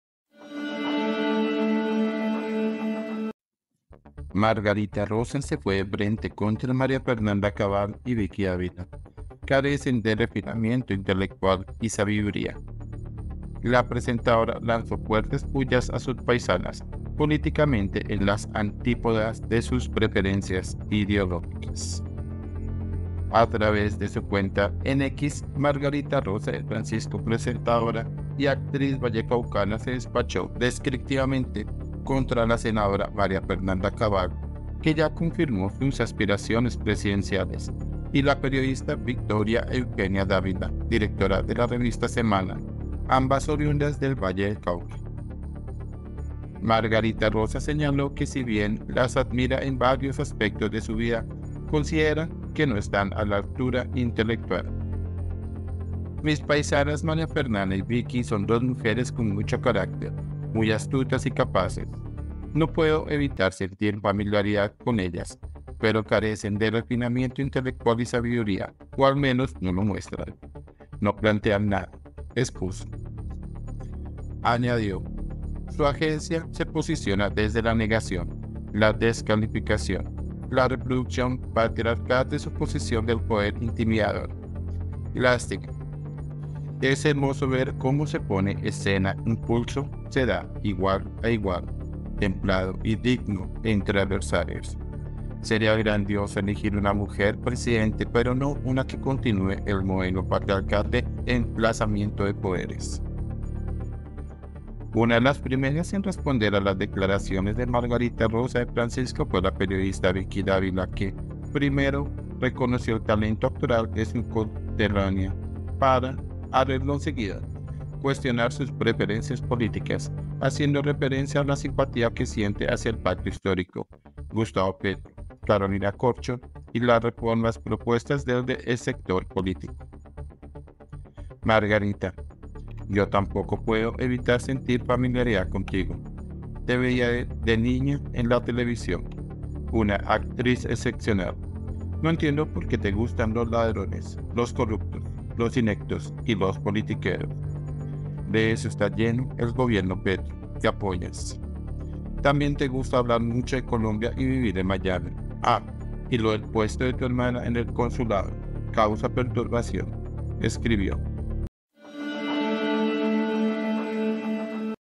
❌ MARGARITA ROSA SE FUE DE FRENTE CONTRA MARÍA FERNANDA CABAL Y VICKY DÁVILA
A través de su cuenta e X, Margarita Rosa de Francisco, presentadora y actriz vallecaucana, se despachó, descriptivamente, contra la senadora María Fernanda Cabal -que ya confirmó sus aspiraciones presidenciales- y la periodista Victoria Eugenia Dávila, directora de la revista Semana, ambas oriundas del Valle del Cauca.
INFOBAE